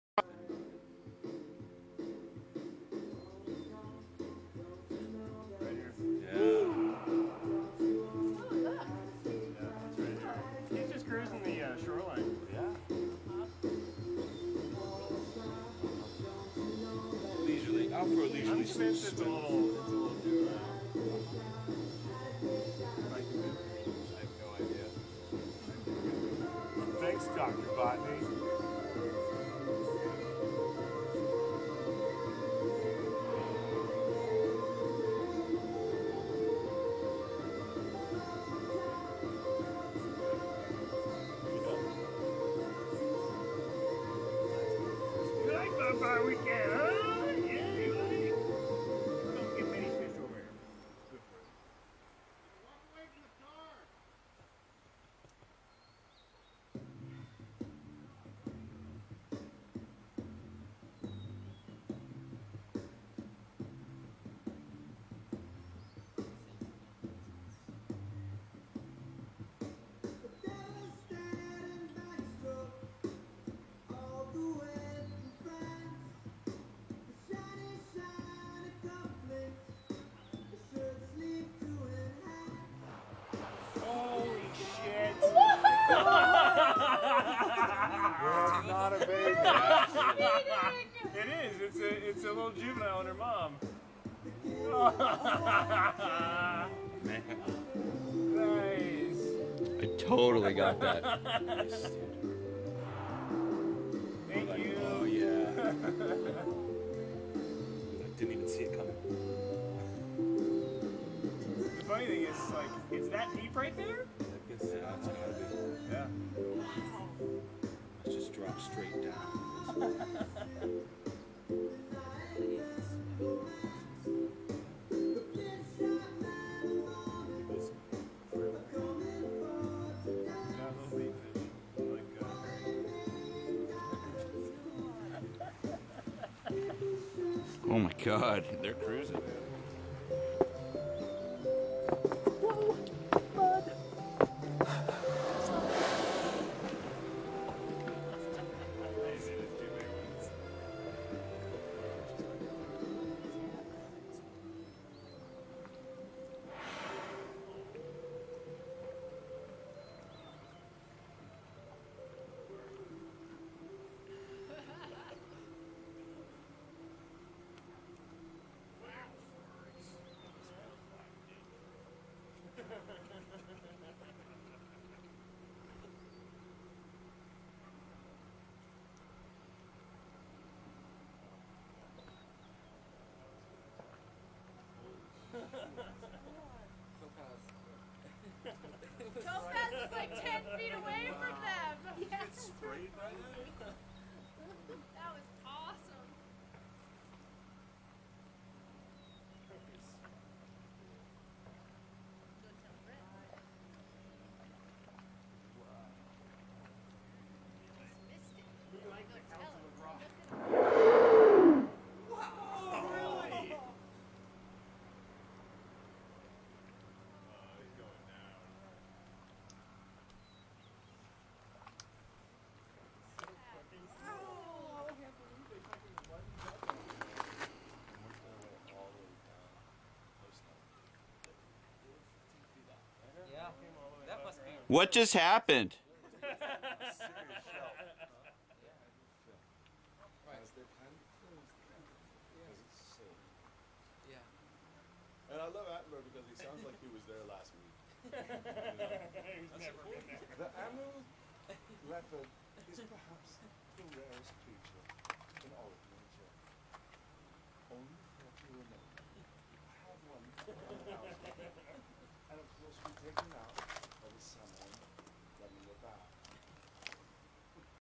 Humpbacks in Lynn Canal
A mama and baby humpback whales swam by us while we were down at the beach on Lynn Canal, Alaska, just outside of Juneau. They came within 10 feet of the rocky shore just around sunset.